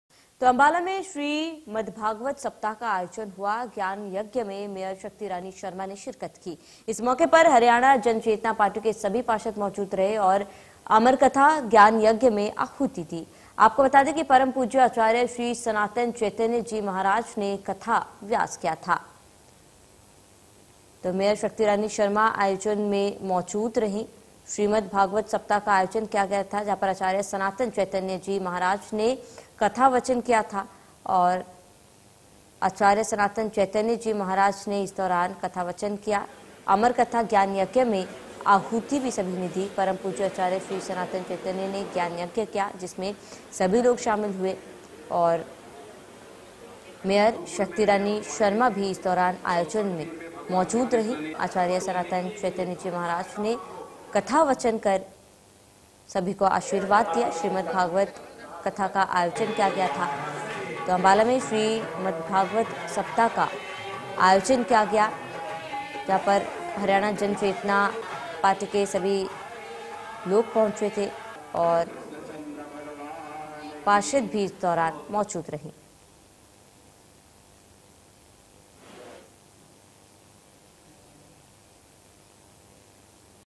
Ambala : श्रीमद् भागवत सप्ताह का आयोजन, मेयर शक्ति रानी शर्मा आयोजन में मौजूद रही
Ambala : श्रीमद् भागवत सप्ताह का आयोजन, मेयर शक्ति रानी शर्मा आयोजन में मौजूद रही  | India News Haryana